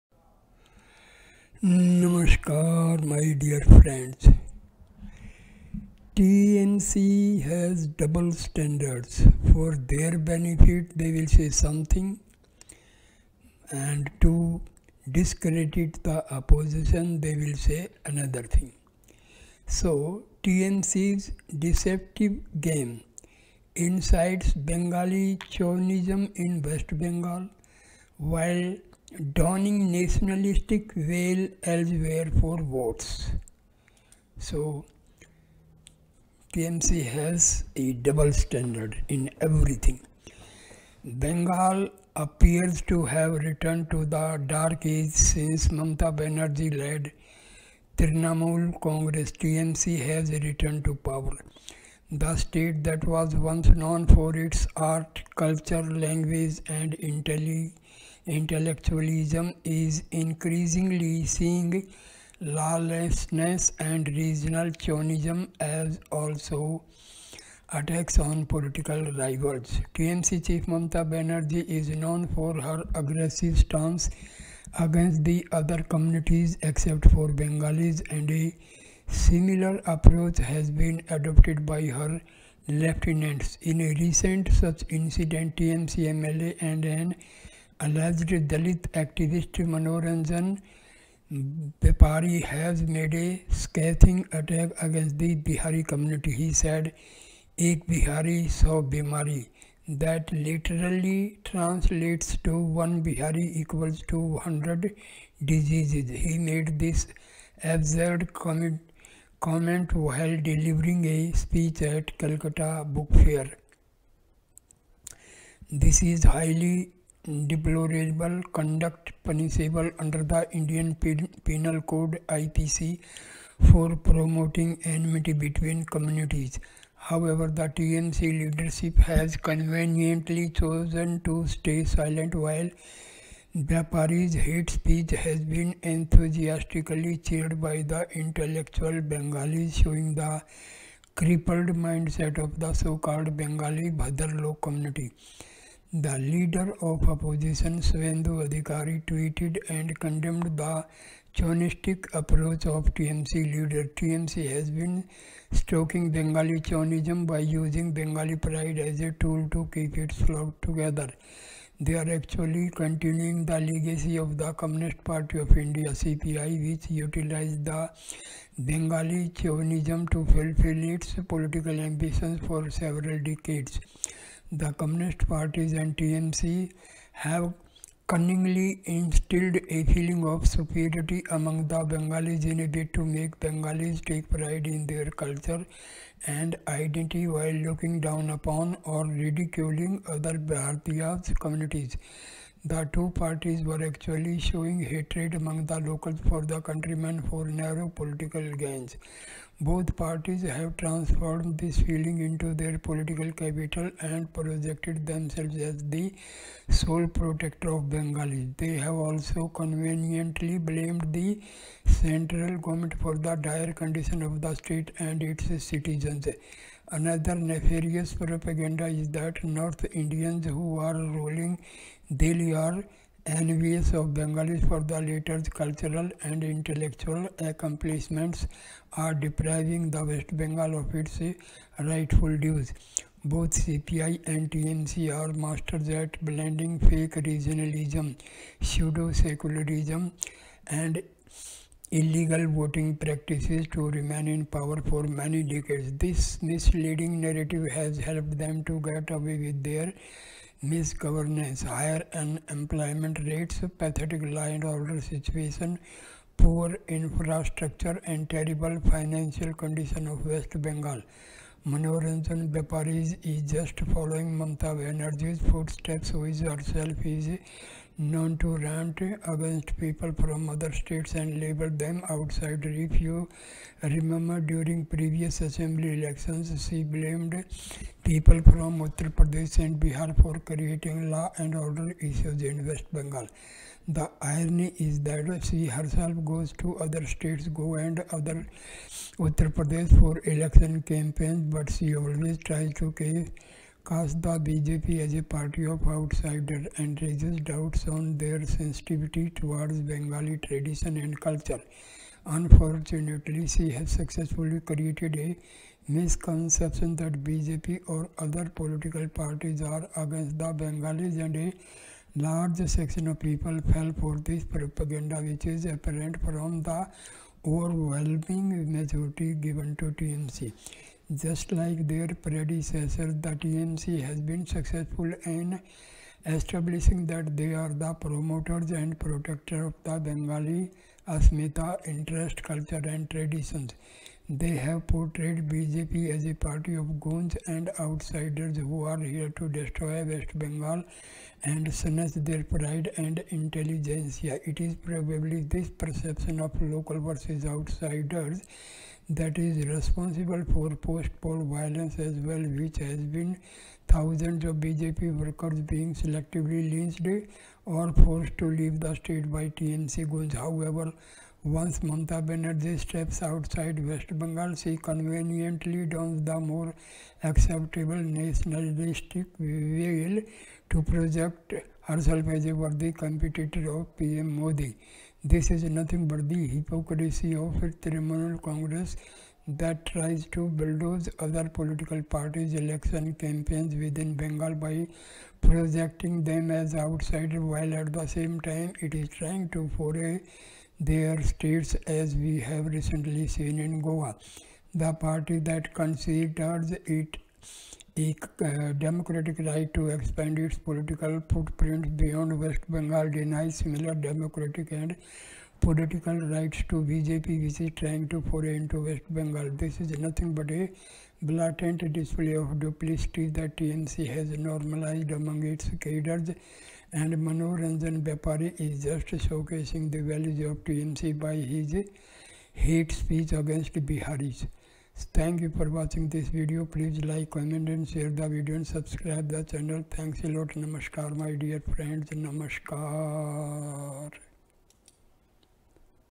TMC’s Deceptive Game- Incites Bengali Chauvinism In WB-Donning ‘Nationalistic’ Veil Elsewhere
Bengal appears to have returned to the dark age since Mamata Banerjee-led Trinamool Congress (TMC) has returned to power. The state that was once known for its art, culture, language, and intellectualism, is increasingly seeing lawlessness, and regional chauvinism as also attacks on political rivals.
    TMC chief Mamata Banerjee is known for her aggressive stance against the other communities except for Bengalis and a similar approach has been adopted by her lieutenants. In a recent such incident, TMC MLA and an alleged Dalit activist Manoranjan Byapari has made a scathing attack against the Bihari community, he said: ‘Ek Bihari Sau Bimari’, that literally translates to ‘one Bihari equals to hundred diseases’. He made this absurd comment while delivering a speech at Kolkata Book Fair.